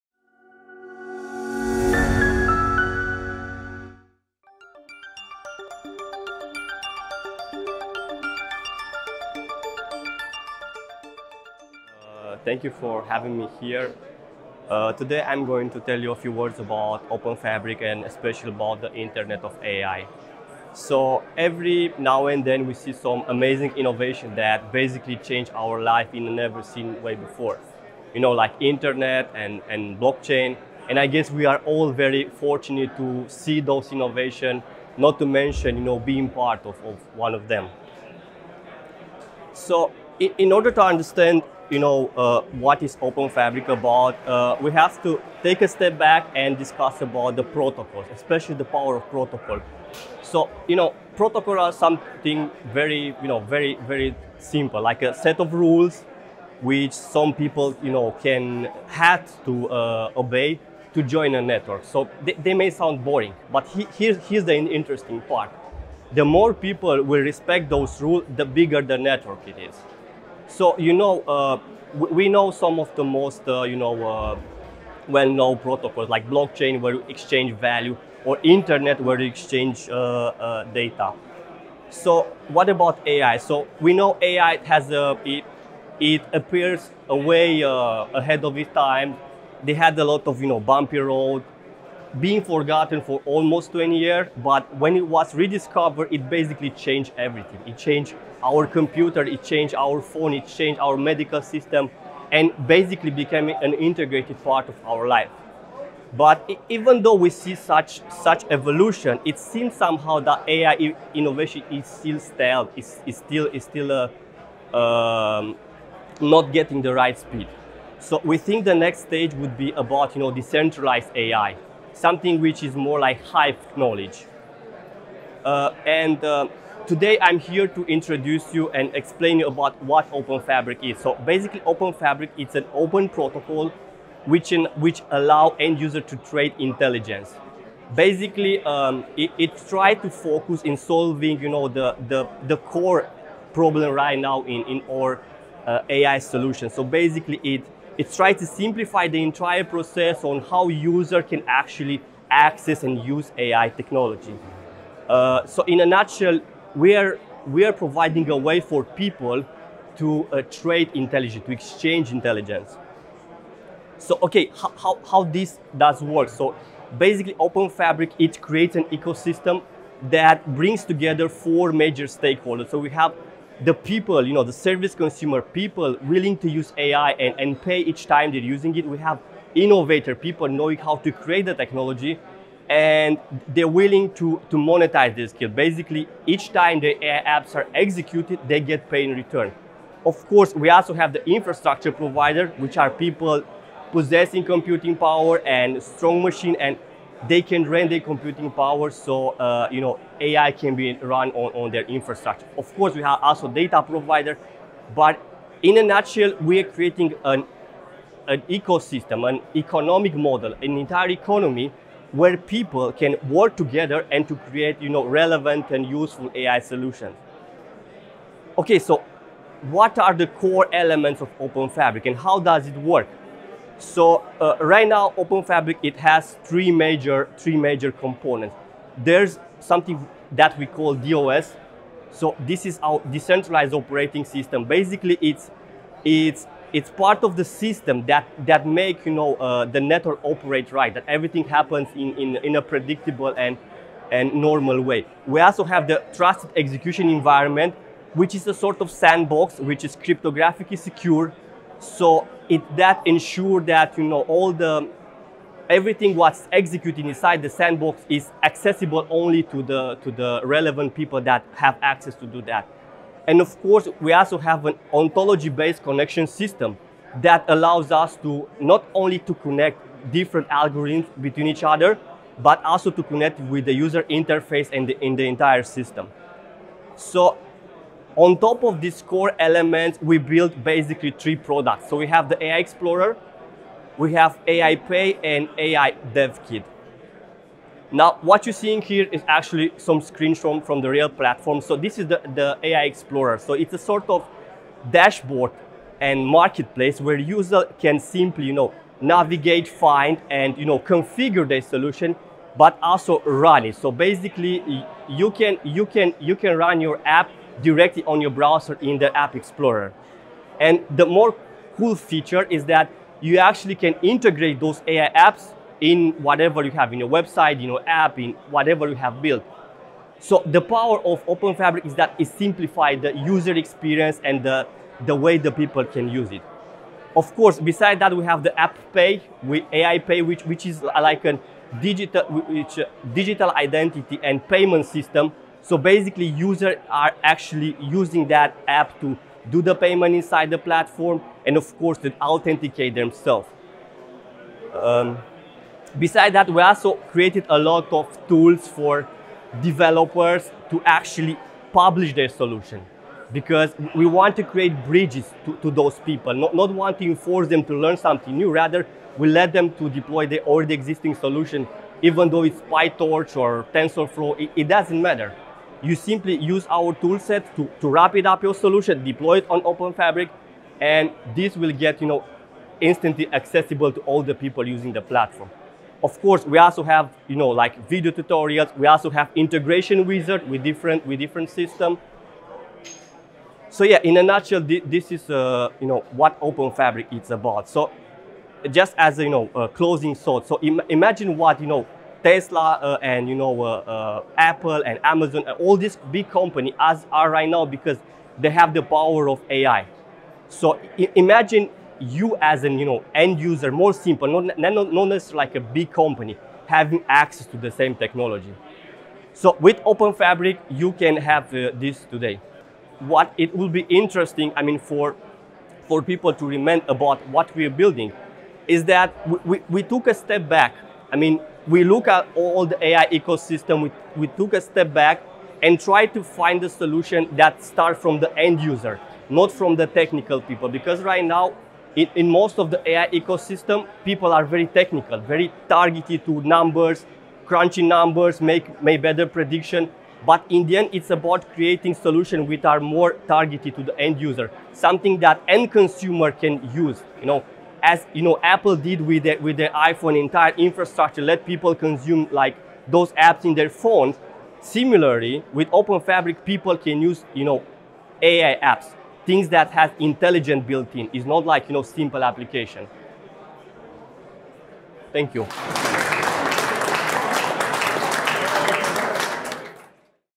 How will people trade intelligence? | Openfabric | The Internet of AI | Berlin
ai is probably one of the most powerful tools ever invented by the human mind. It updated our cars, phones, computers, and medical devices, essentially becoming an integrated part of our lives. Even though we have made significant progress, the technology remains exclusivist, expensive, fragmented, dominated by giants and super hard to use and understand.

We learned that, for ai to evolve, it doesn't need only more powerful computers.
It needs connections and wide usage, and we know protocols are an excellent tool for that.

Today we are introducing a revolutionary product that resulted in more than two years of research, design, and experimentation.

Openfabric utilizes the latest advancements in blockchain, advanced cryptography, and infrastructure to create a new medium where
and algorithms can connect in a new way never seen before, forming a unique ecosystem.

It solves the most complicated problems faced by AI platforms today:
- It reduces the infrastructure need and technical know-how
- It ensures scalability at a reduced cost
- It eliminates vendor lockup
- It creates new market opportunities for everybody

The Internet will never be the same.

Join the new Internet of AI.